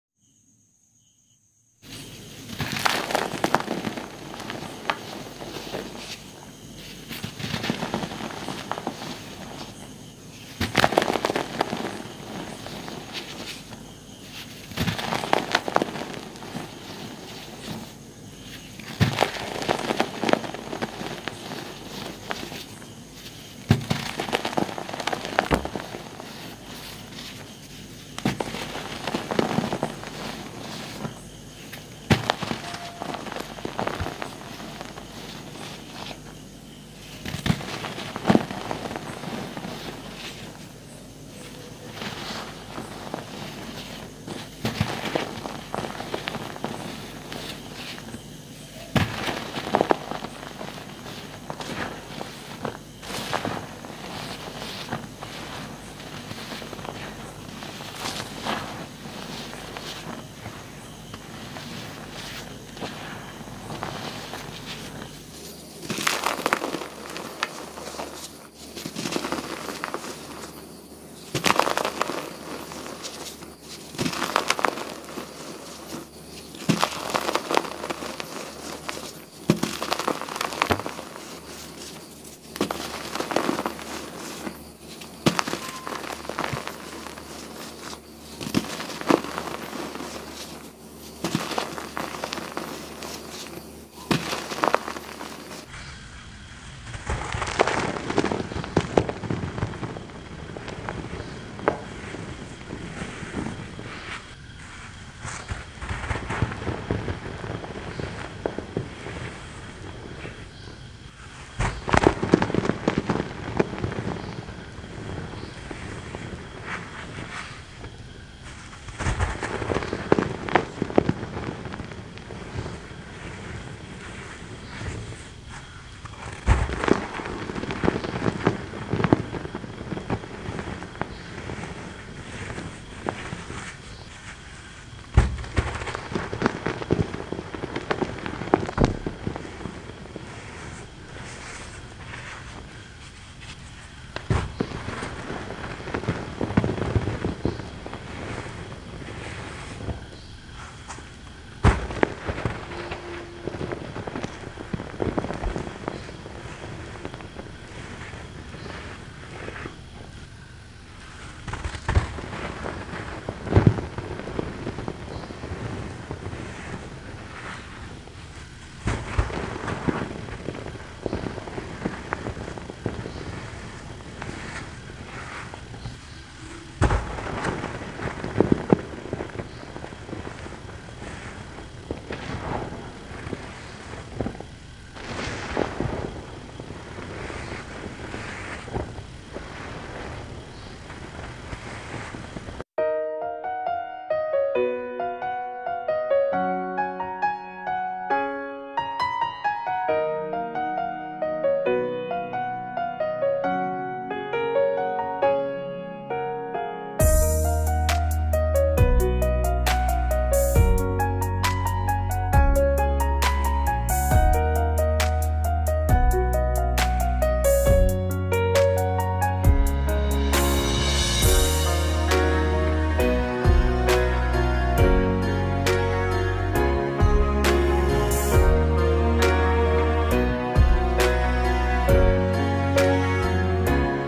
Gym Chalk Holipowder Bomb Series// Blue//Blocks Crush//ASMR //oddlysatisfying
hiii everyone 🖐

so I have started holipowder  color "BOMB" series hope you all enjoy this Crush

for this series added 4 sessions
normal crush,fast speed, slow motion, behind the scenes

crush starts o:o2
fast speed at 1:05
slow motion at 1:35
behind the scenes 3:12